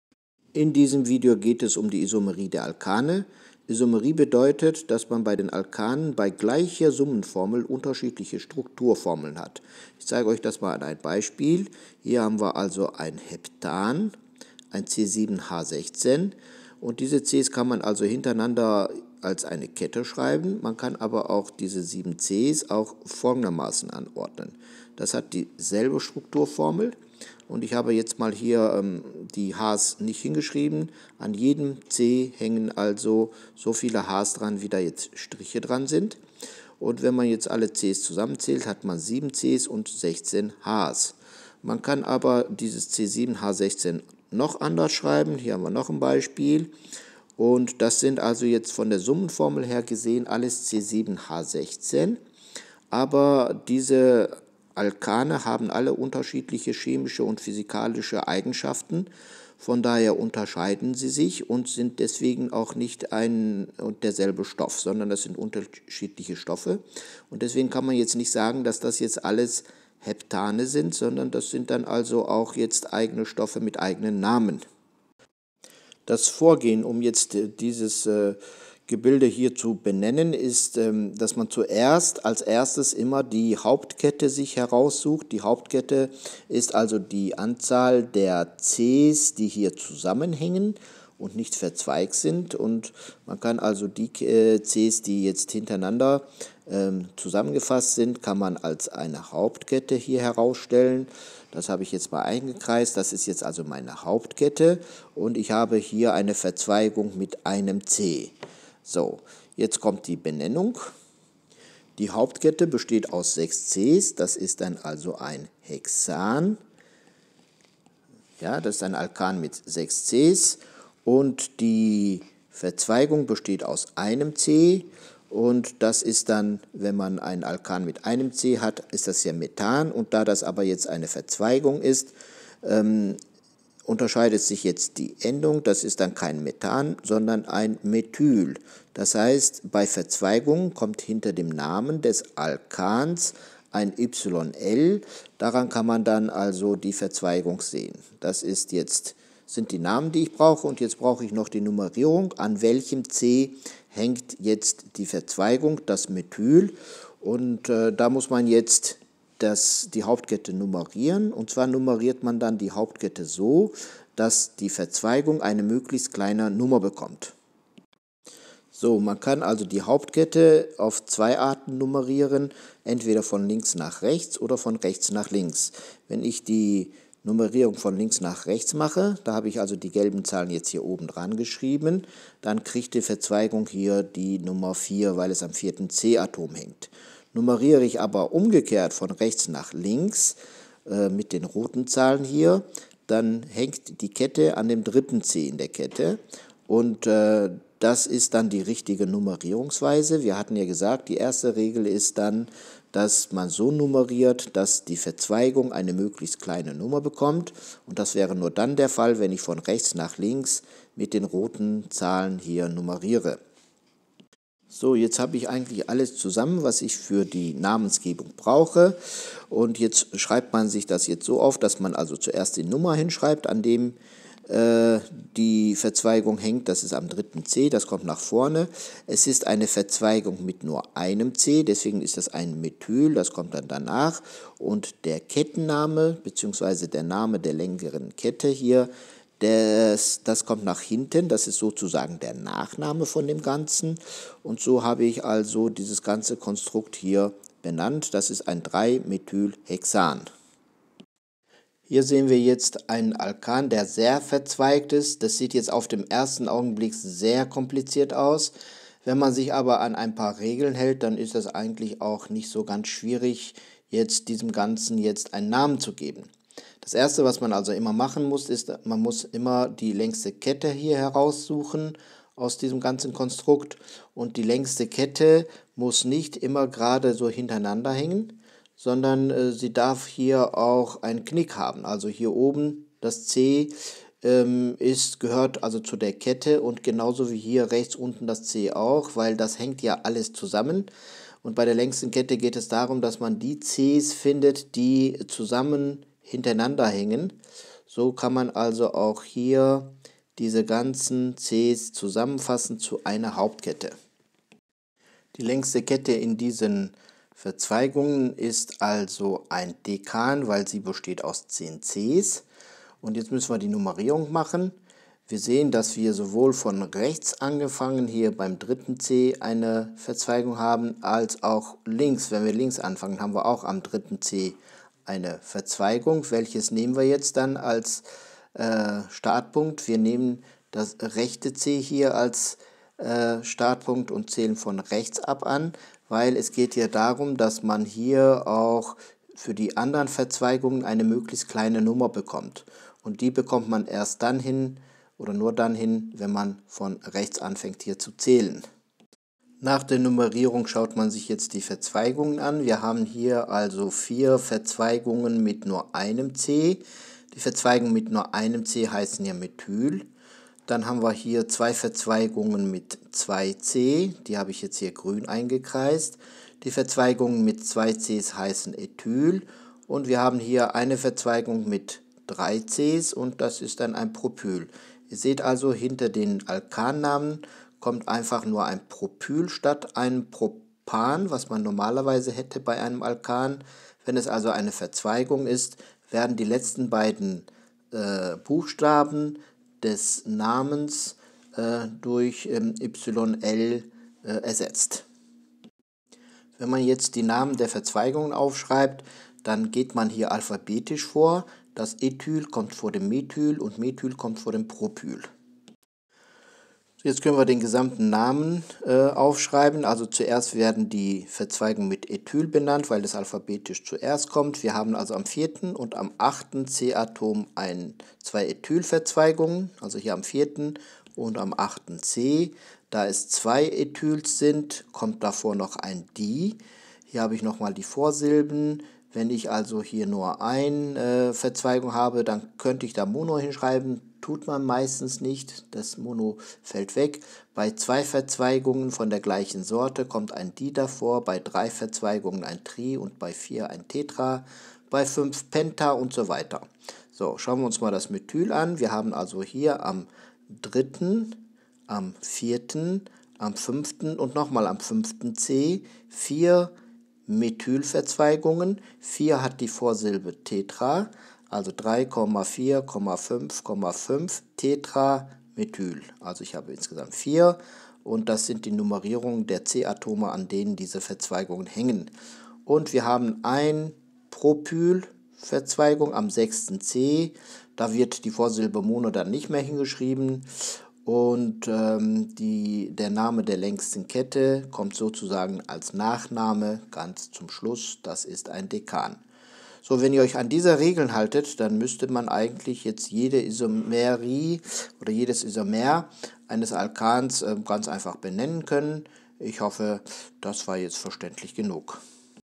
Alkane benennen
Verzweigte Alkane richtig benennen.

Halogenalkane - Was sind Halogenalkane? Nomenklatur der Halogenalkane